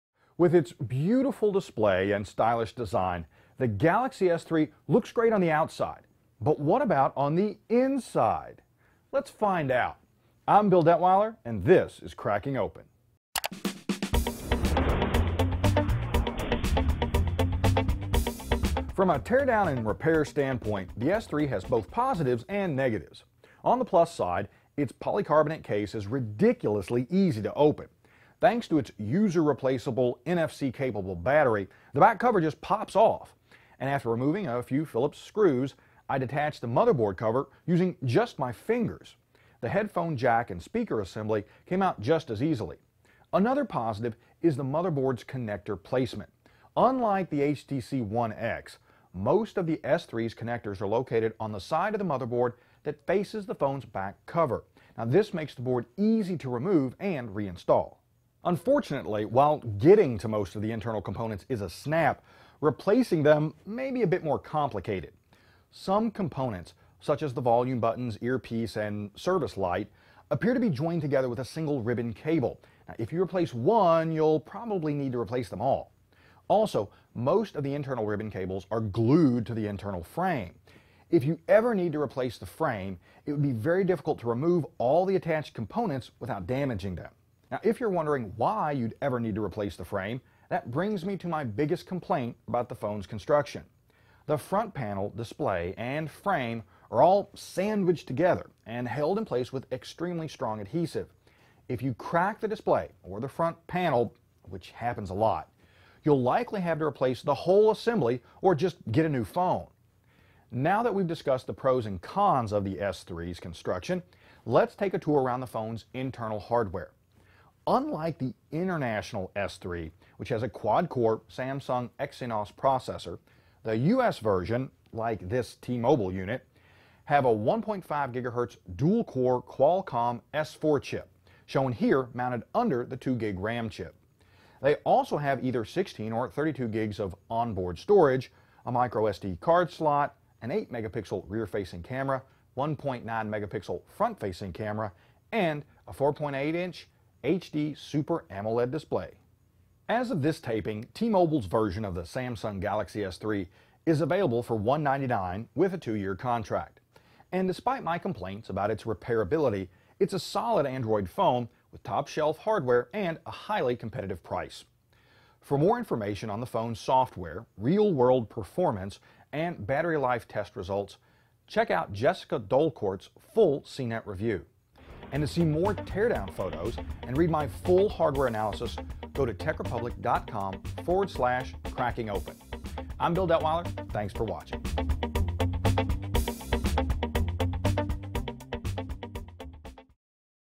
Cracking Open - Samsung Galaxy S III (T-Mobile)
The Galaxy S III is to crack open, but Bill Detwiler explains why replacing the screen or front panel will be costly.